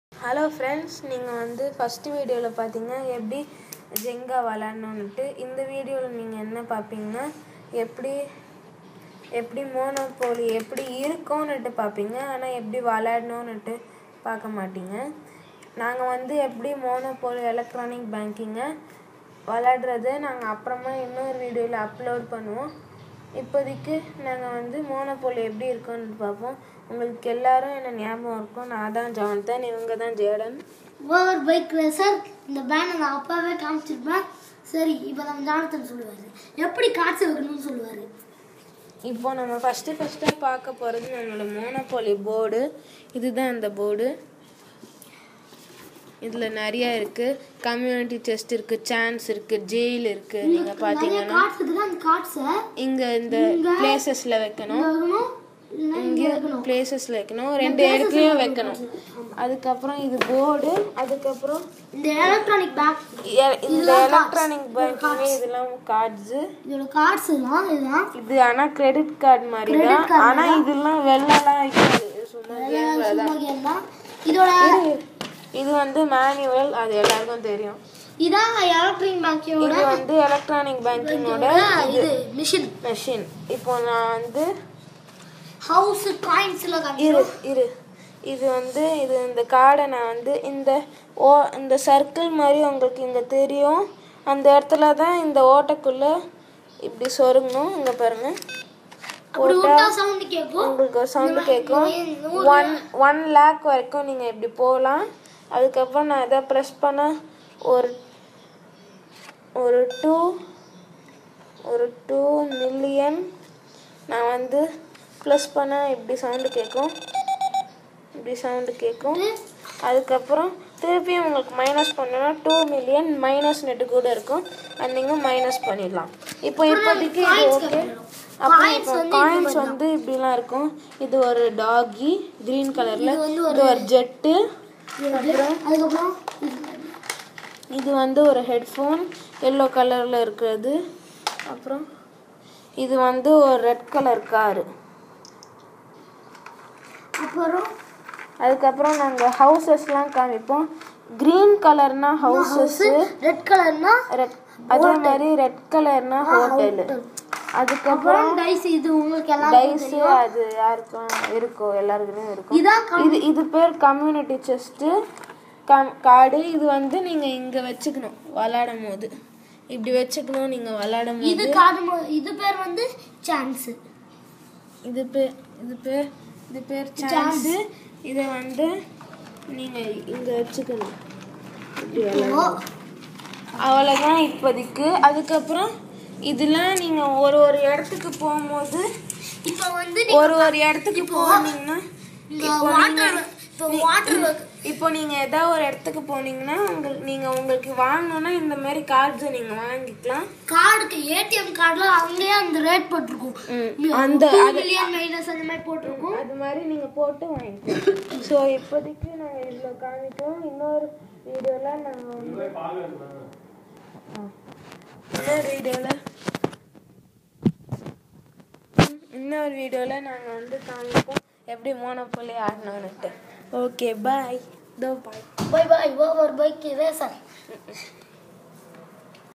What is inside Monopoly electronic banking game?
See in this video what is inside monopoly electronic banking game